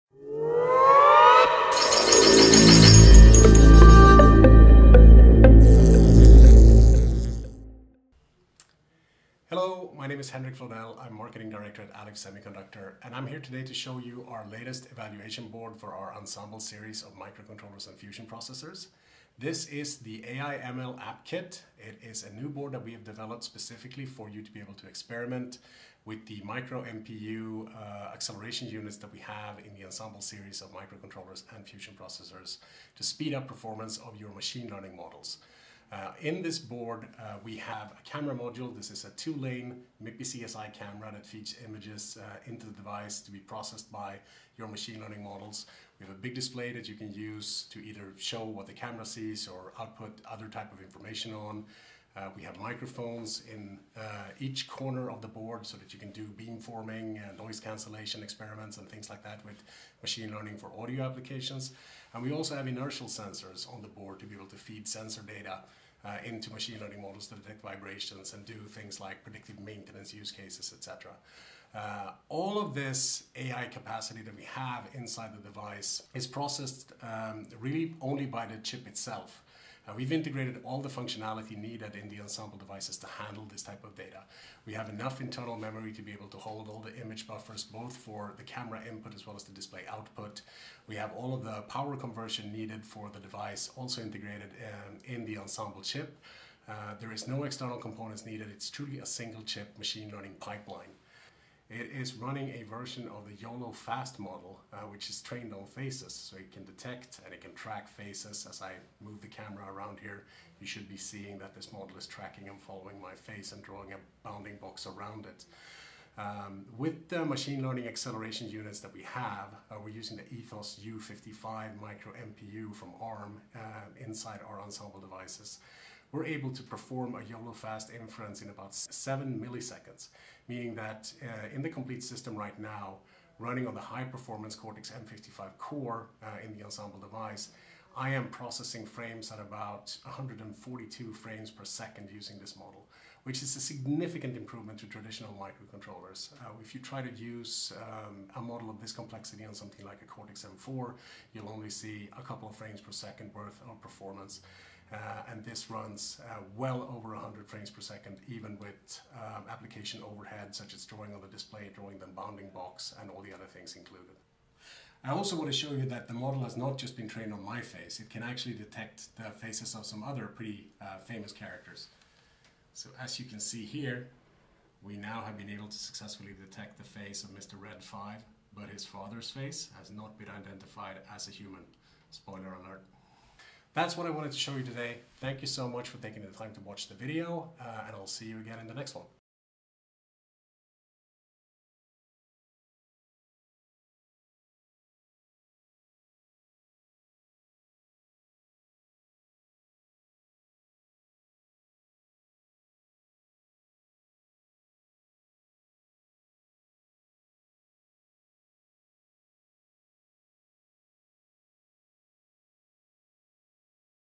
40X Faster Face Detection
Henrik Flodell, Senior Director of Marketing at Alif Semiconductor, is demoing an AI-Enhanced Face Detection Demo powered by Alif Ensemble E3, with 7 msec per inference operation, which is 40X faster than the conventional MCU. More details below:

- Image data buffered in on-die SRAM
- YOLO face detection model stored in on-die MRAM
- AI/ML inference via Cortex-M55 MCU Core & Ethos-U55 microNPU
- AI/ML model operation results visualized
- Graphic frame buffers in on-die SRAM
- Images rendered with face tracking attributes
- 24bpp color TFTLCD panel driven by MIPI-DSI
- Results transmitted wirelessly using Telit WE310FS WI-FI/BLE Combo module

NOT Required:
- External memories, power management, or secure element. This is all included in Alif Ensemble devices.